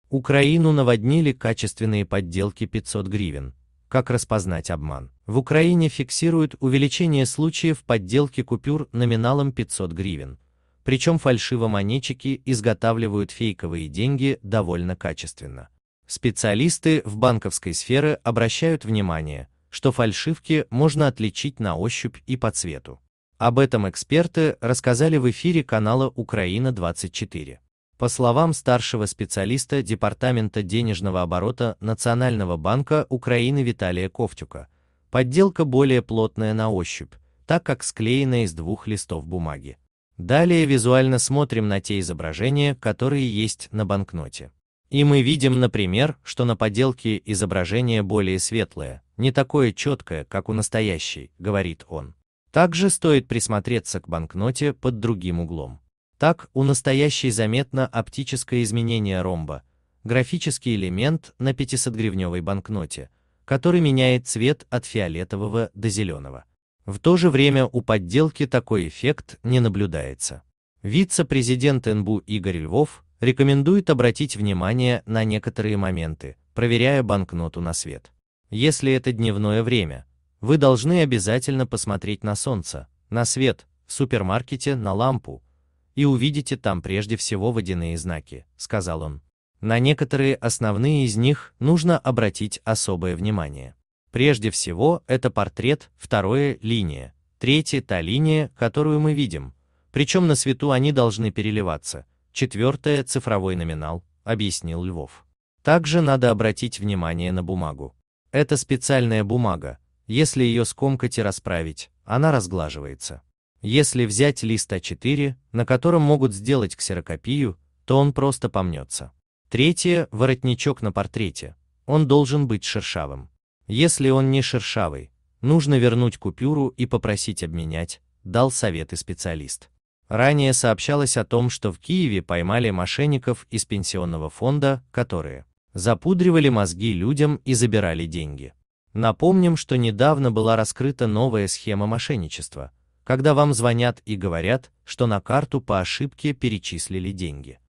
Украину наводнили качественные подделки 500 гривен: как распознать обман.
Новости Жизнь деньги фальшивомонетчики мошенничество банкнота Гривна В Украине фиксируют увеличение случаев подделки купюр номиналом 500 гривен, причем фальшивомонетчики изготавливают фейковые деньги довольно качественно. Специалисты в банковской сферы обращают внимание, что фальшивки можно отличить на ощупь и по цвету. Об этом эксперты рассказали в эфире канала "Украина 24". По словам старшего специалиста департамента денежного оборота Национального банка Украины Виталия Ковтюка, подделка более плотная на ощупь, так как склеена из двух листов бумаги. "Далее визуаль